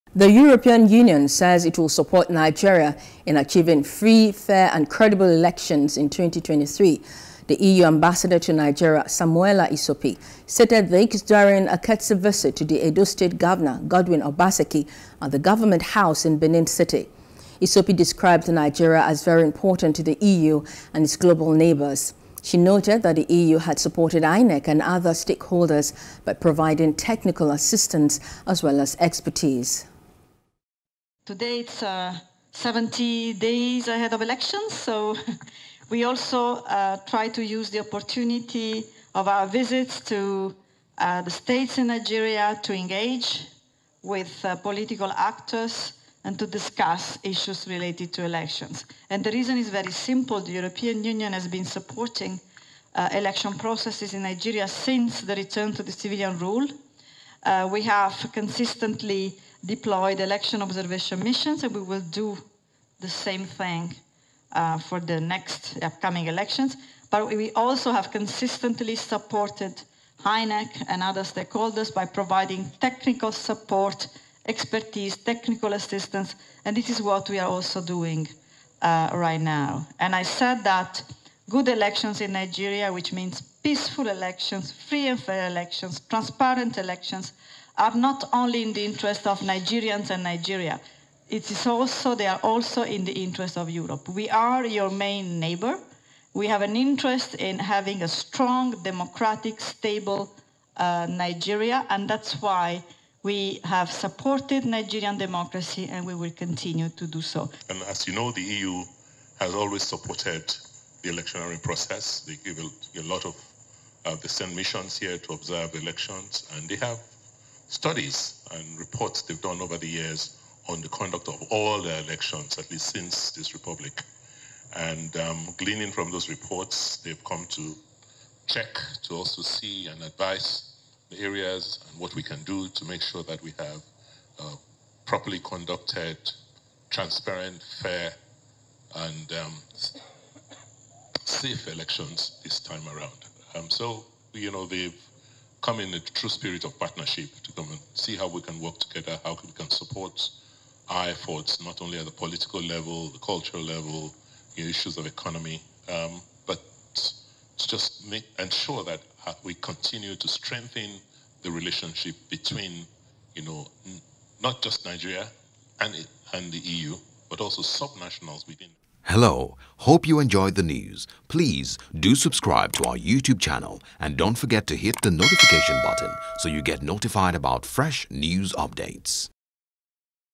2023 Elections: E.U. Pledges Support For Credible Polls
The European Union, EU says it will support Nigeria in achieving free, fair and credible elections in 2023.